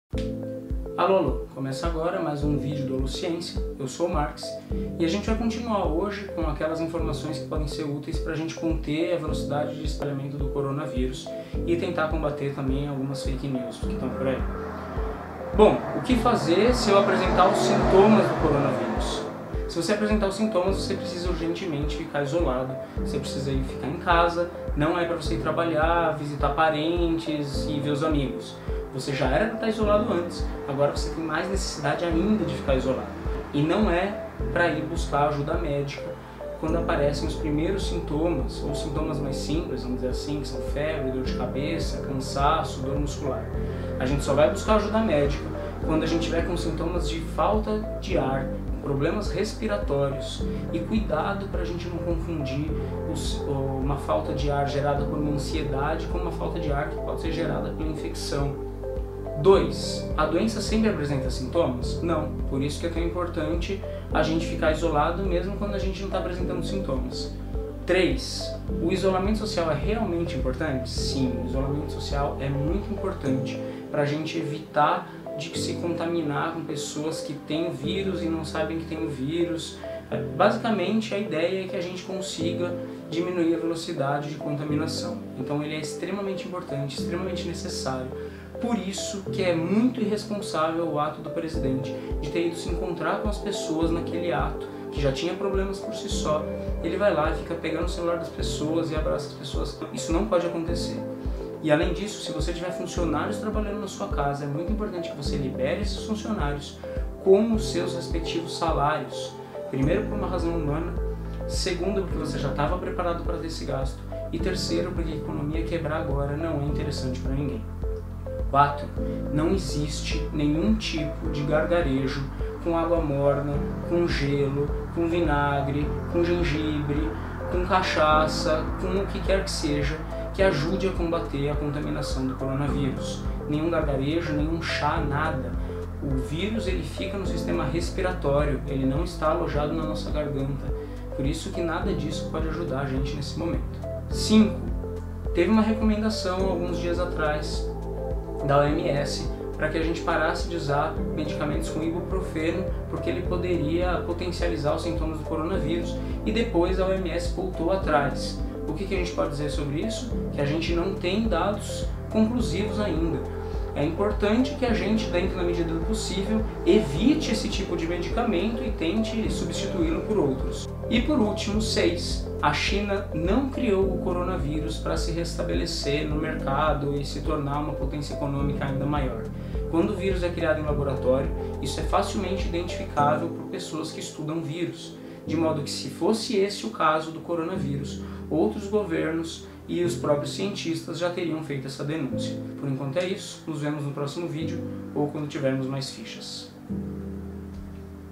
Coronavírus - Notícias falsas e informação - 3 | 006
Alô, alô! Vamos ajudar a espalhar informação de qualidade e combater notícias falsas sobre o coronavírus.

CRÉDITOS
Apresentação, pesquisa, roteiro e edição: Marco Marx